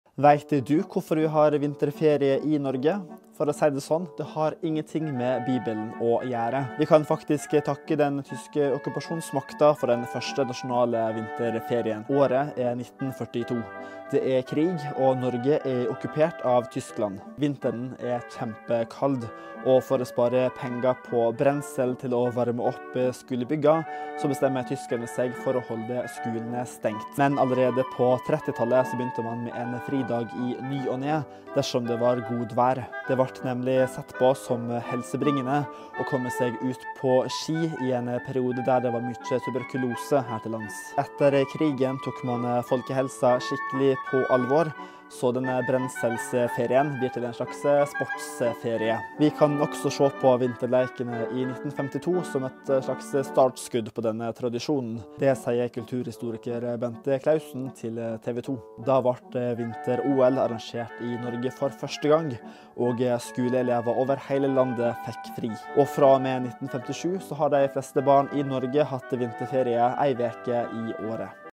Derfor har vi vinterferie
TAKK TIL TYSKERNE: For å si det sånn, vinterferien har ingenting med bibelen å gjøre.